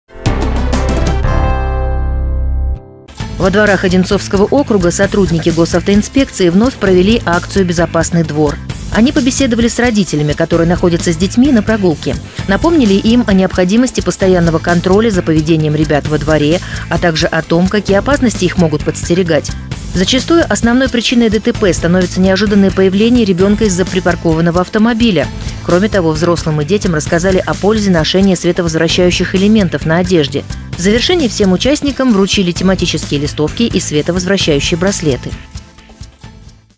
Безопасный двор
Во дворах Одинцовского  округа сотрудники Госавтоинспекции вновь провели акцию «Безопасный двор».  Они побеседовали с родителями, которые находятся с детьми на прогулке. Напомнили им о необходимости постоянного контроля за поведением ребят во дворе, а также о том, какие опасности их могут подстерегать.